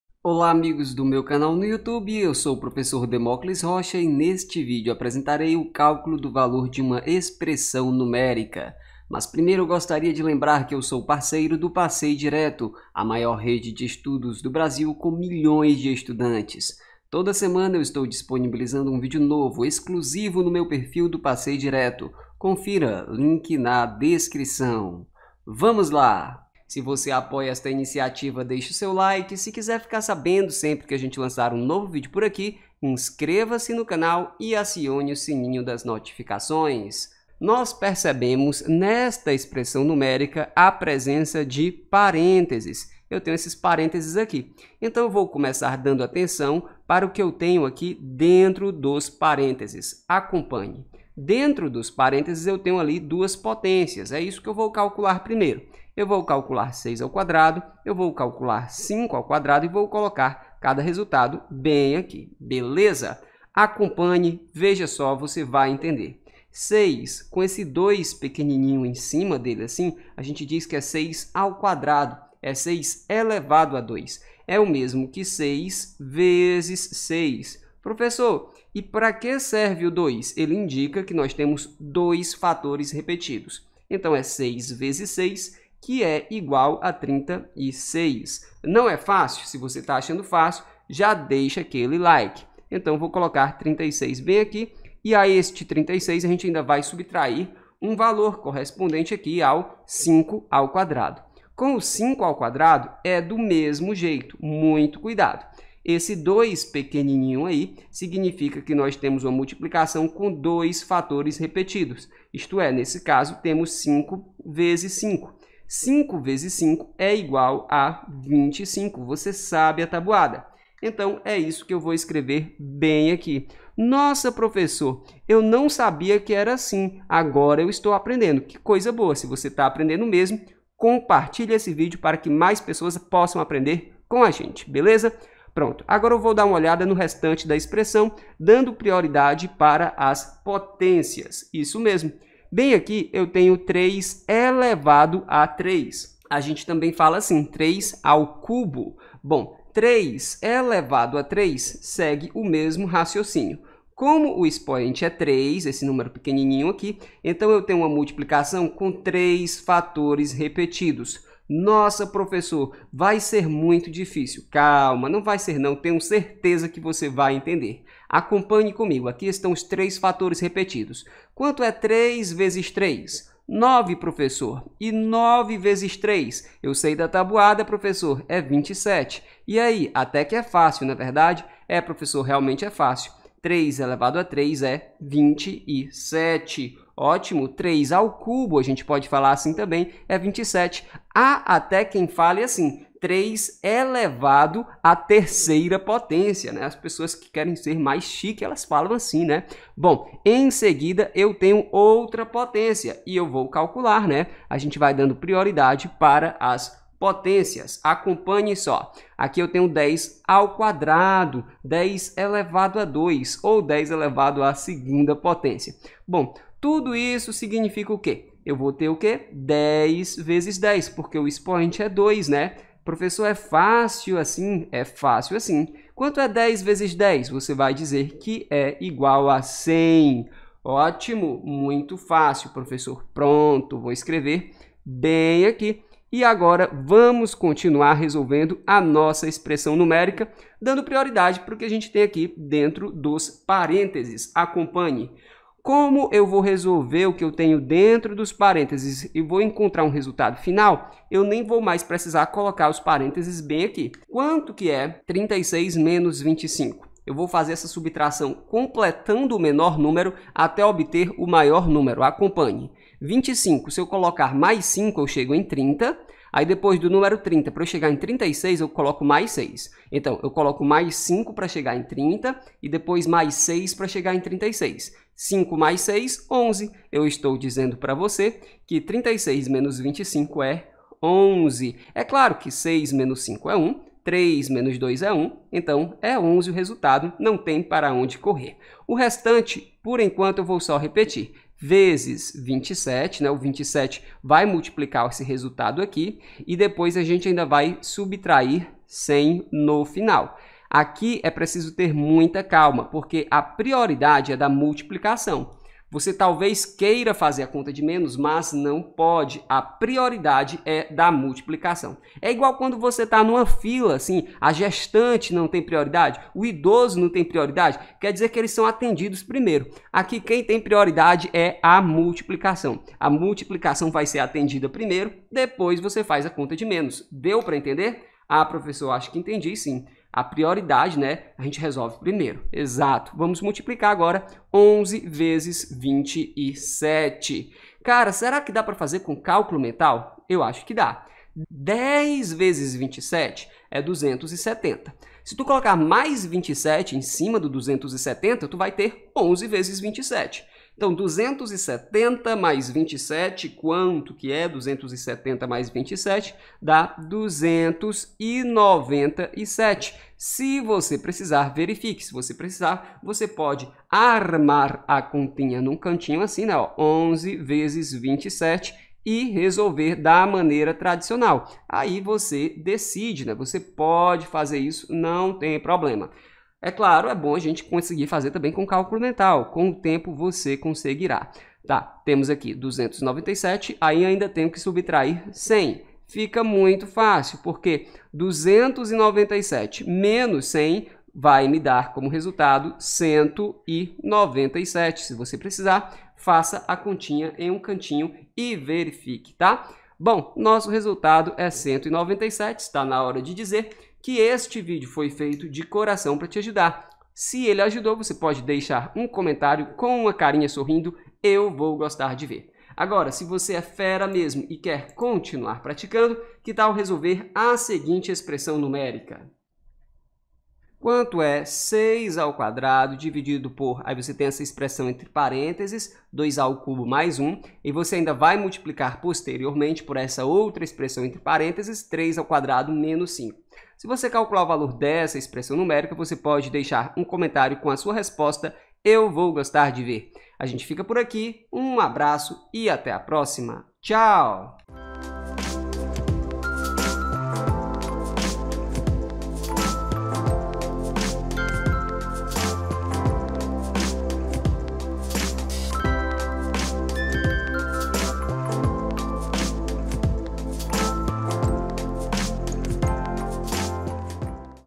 EXPRESSÃO NUMÉRICA
Olá, amigos! Neste vídeo apresento o cálculo do valor de uma expressão numérica

Resposta do exercício proposto: 6² / (2³+1)*(3² - 5) = 16